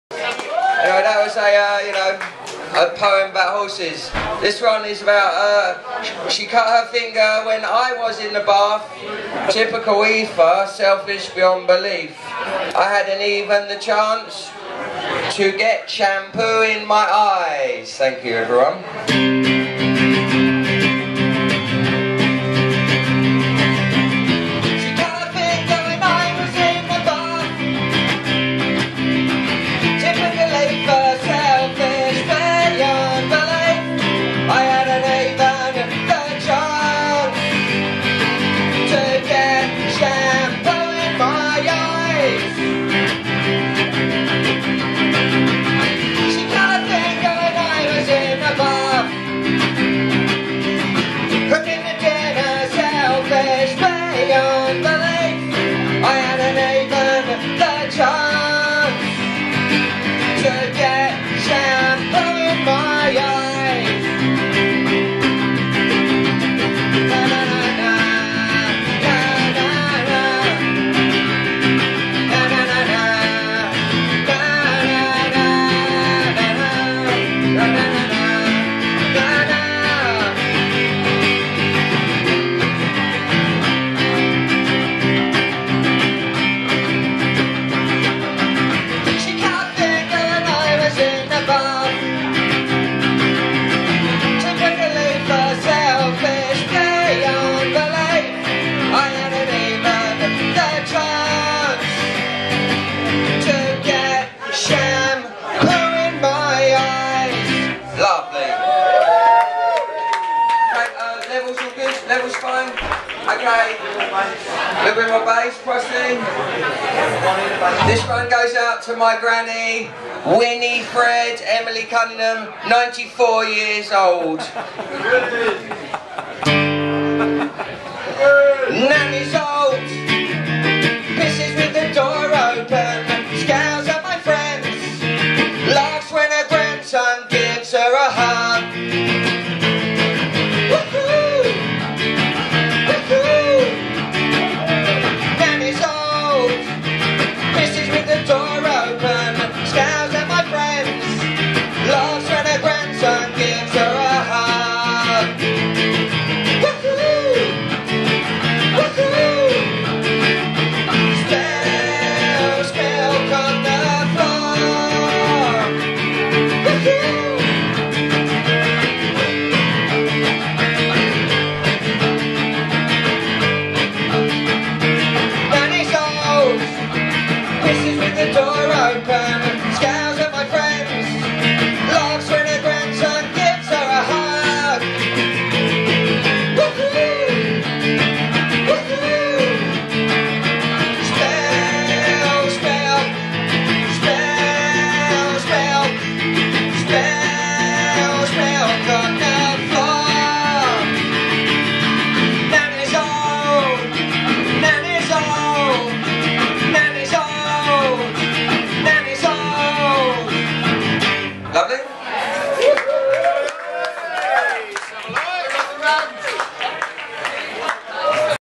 Wonk Unit acoustic set at Trash'd New Wave Festival 2015
Wonk Unit gave up a support slot with Slaves to play an acoustic set at Trash'd New Wave Festival 2015 opening night at the Crown public Hastings - the private view of Do It Yourself, an exhibition of punk photography by Mark Richards. Thanks guys!